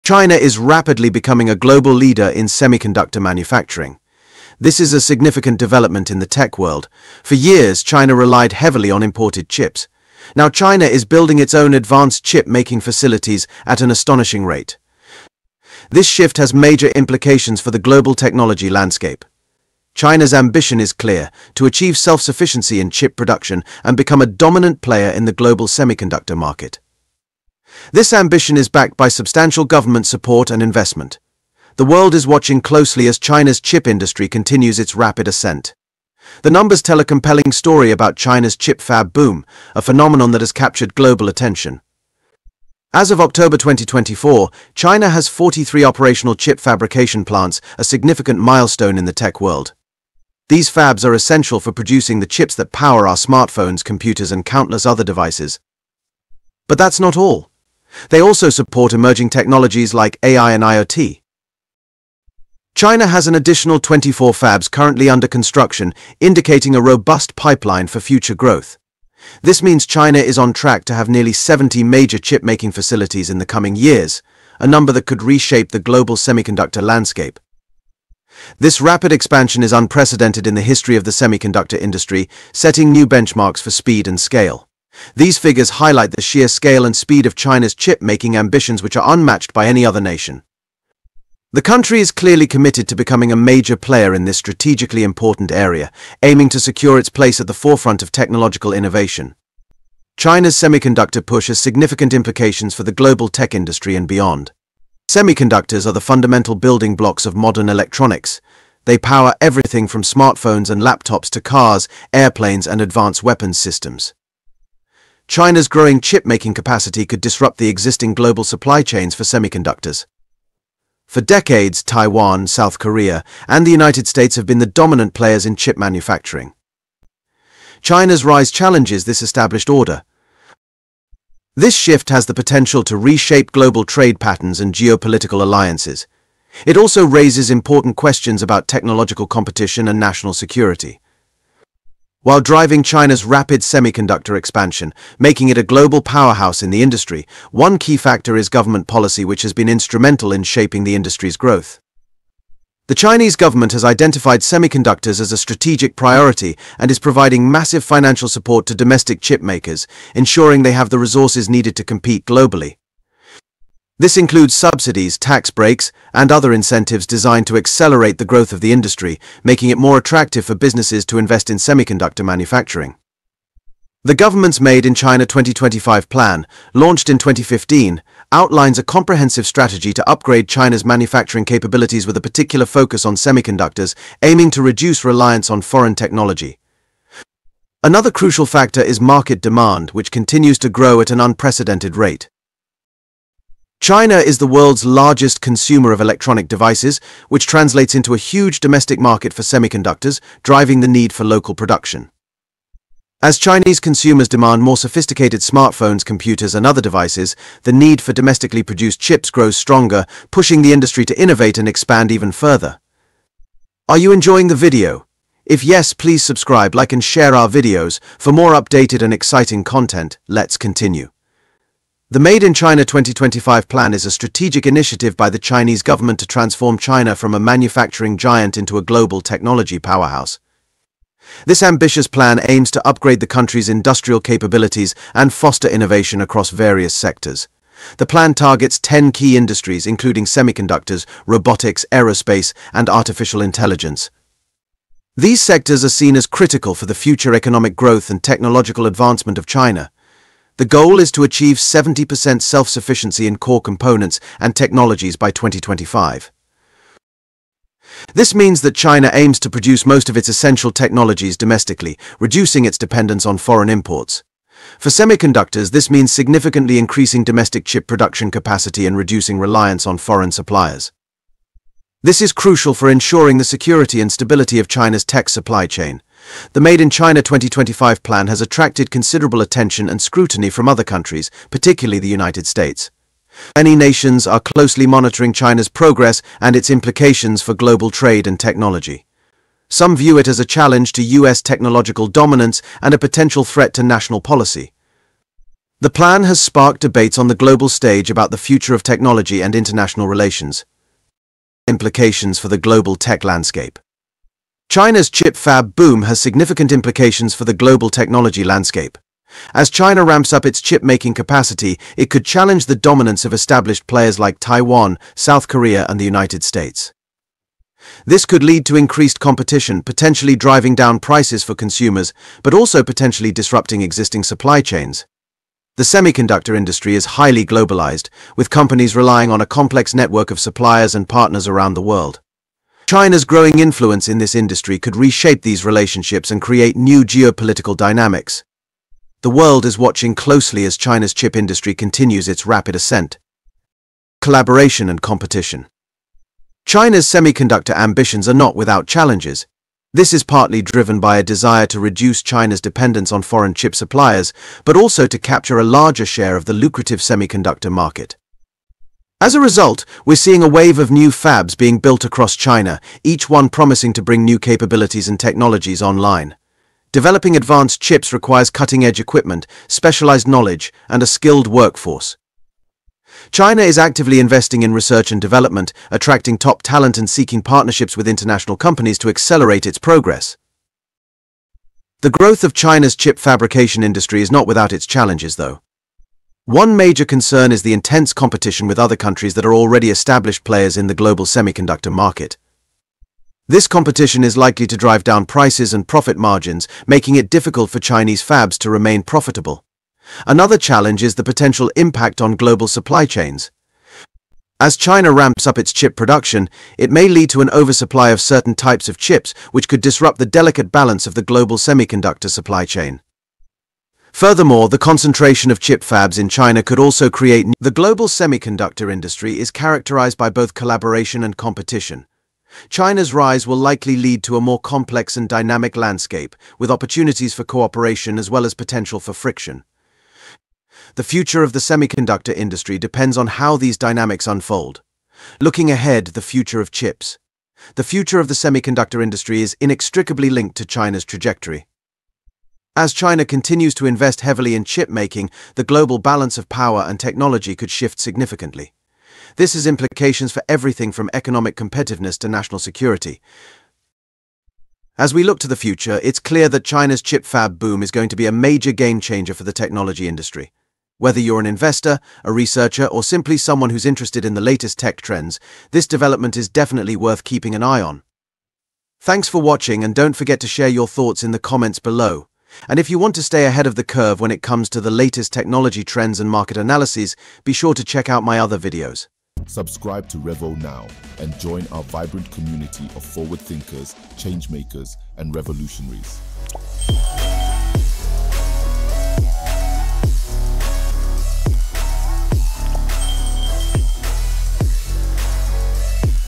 China's CHIP FAB Boom: 43 Operational and 24 MORE Coming!
China is rapidly emerging as a major player in the global semiconductor industry, with a whopping 43 operational chip fabrication plants and 24 more in the pipeline! In this video, we'll delve into the reasons behind China's chip fab boom, the implications for the global tech landscape, and what it means for the future of the industry. From the role of government support and investment to the rise of domestic chipmakers, we'll explore it all. So, let's dive in and find out what's driving China's chip fab frenzy!